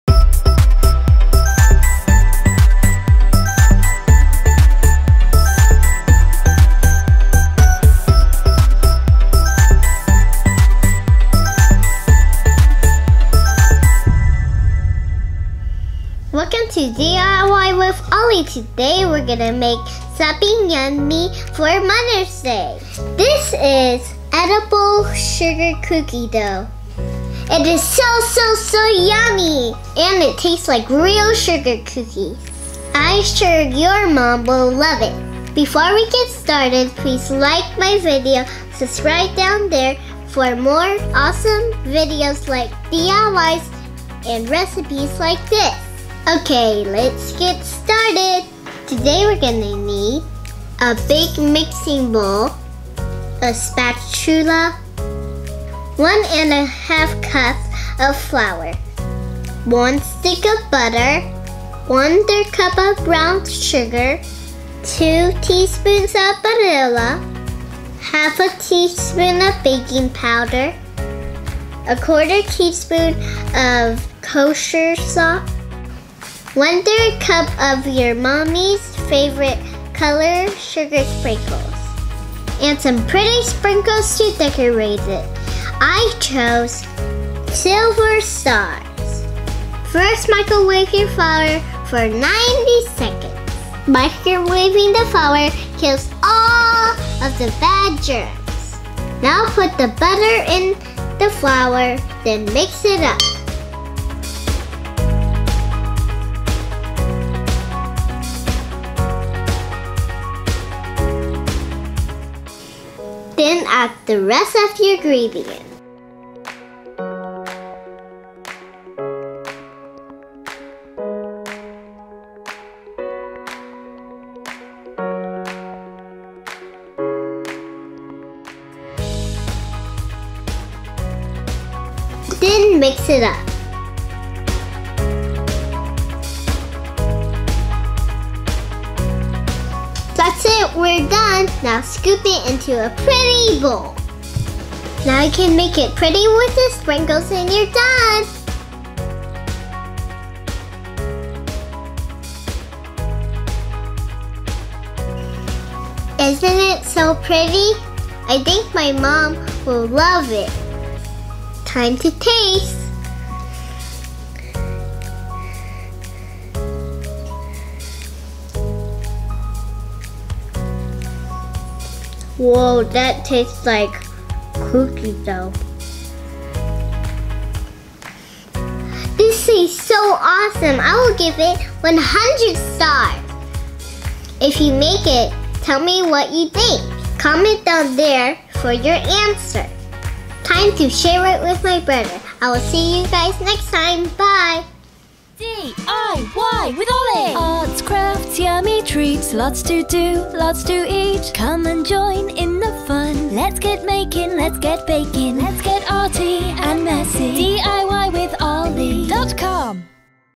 DIY Edible Sugar Cookie Dough | The Best Edible Cookie Dough Recipe Ever! | Mother's Day Gift Ideas
This is the BEST Edible Cookie Dough Recipe you'll ever make! It tastes just like a real freshly baked sugar cookie. Make this DIY Edible Sugar Cookie Dough for your mommy on Mother's Day! You can put the edible cookie dough in a cute jar and tie it up with a super pretty ribbon. Make sure to buy sugar sprinkles in her favorite color. I'm super duper sure she'll love it (and you will too!)

♡ Ollie